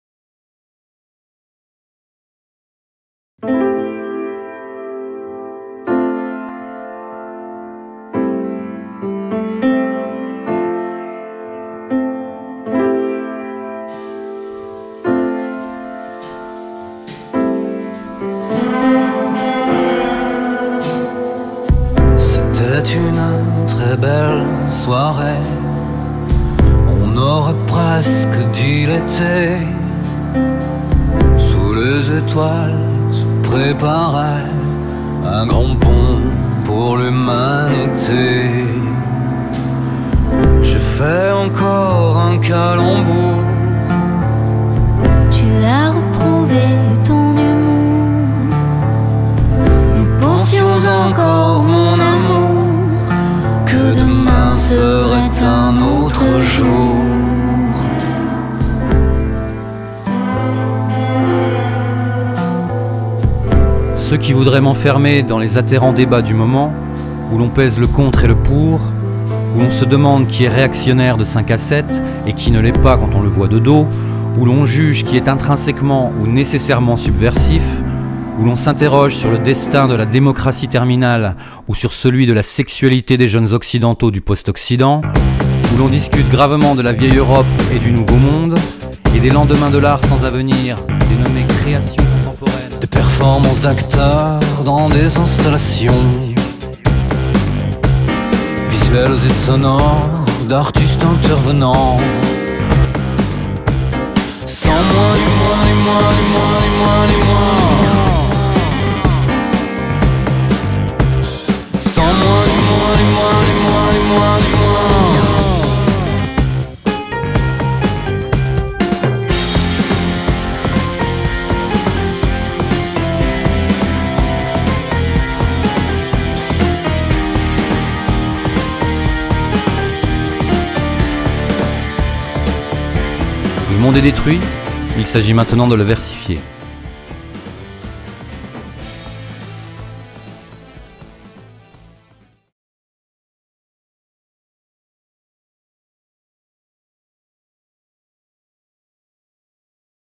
SANS MOI (Philippe Muray/Bertrand Louis) Teaser du spectacle
SANS MOI
Concert lecture
D'après Minimum Respect de Philippe Muray
Conçu et interprété par Bertrand Louis

Piano et voix : Bertrand Louis
Guitare électrique : Jérôme Castel

Direction artistique : Isabelle Censier et Stephan Kalb
Lumières : Jean-luc Chanonat
Son : François Turpin

CRÉATION AUTOMNE 2014

Production et Contact

Compagnie des Petites Heures
Frédéric Biessy / Frédéric Rousseau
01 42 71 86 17

Danagroup
Isabelle Censier / Stephan Kalb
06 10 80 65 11